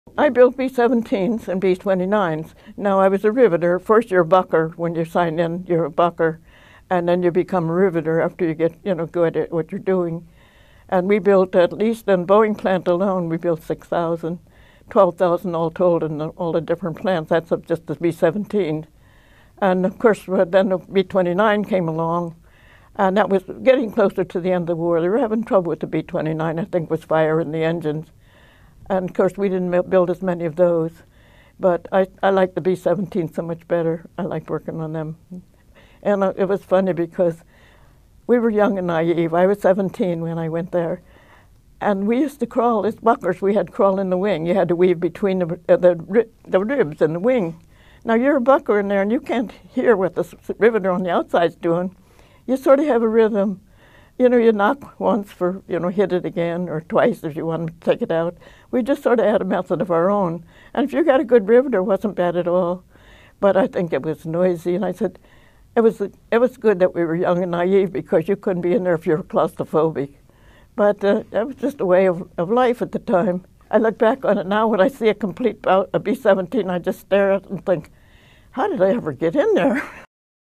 Rosie the Riveter Character Inspired Many
“Rosie the Riveter” was an iconic World War II poster character showing a female riveter flexing her muscle. Mae Krier was the real deal; she describes what it was like building aircraft for the war effort.